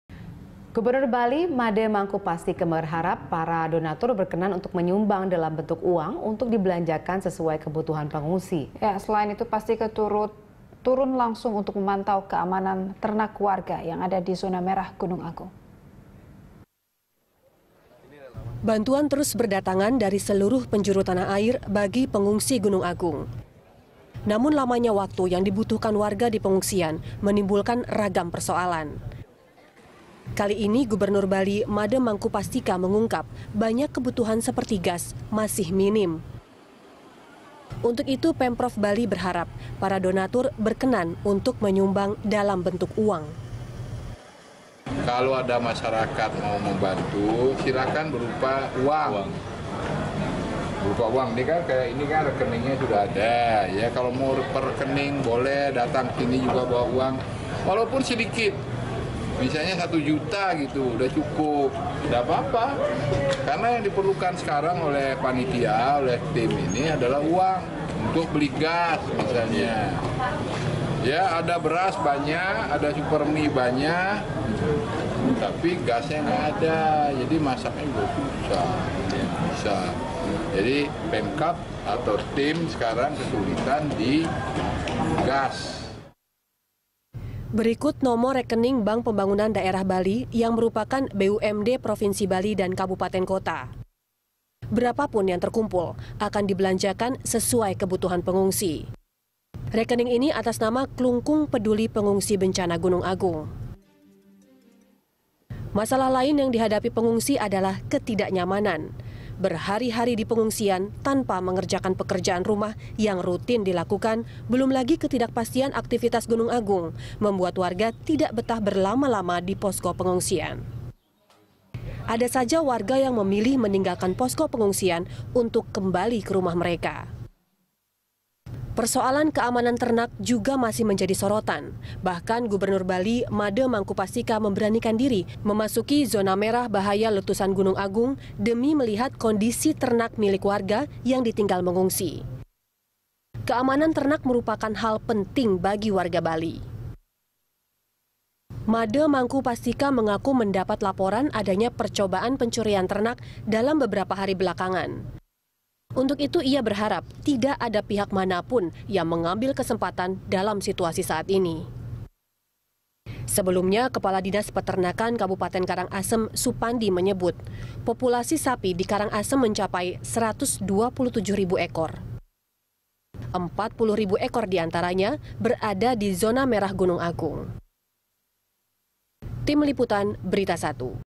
Gubernur Bali Berharap Donatur Menyumbang Dalam Bentuk Uang
Gubernur Bali Made Mangku Pastika berharap donatur berkenan menyumbang dalam bentuk uang untuk dibelanjakan sesuai kebutuhan pengungsi. Selain itu, Pastika turun langsung memantau keamanan ternak warga yang ada di zona merah Gunung Agung.

Facebook.com/BeritaSatuTV
Youtube.com/BeritaSatu